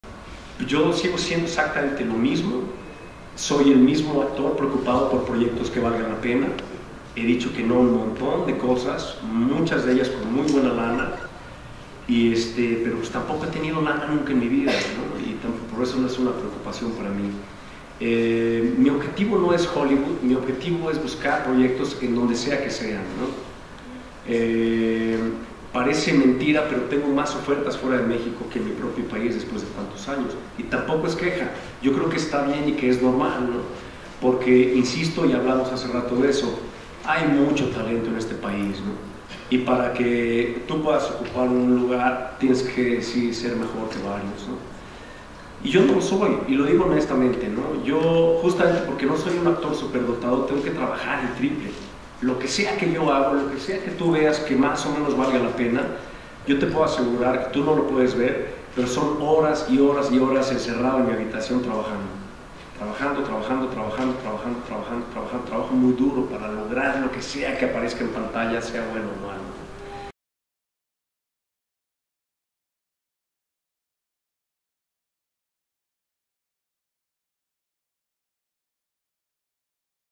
Demian Bichir rumbo al Oscar
Demian Bichir nos da una muestra muy sincera de humildad diciéndonos como cambió su situación personal al estar nominado para un premio tan importante en el mundo del cine como es el Oscar.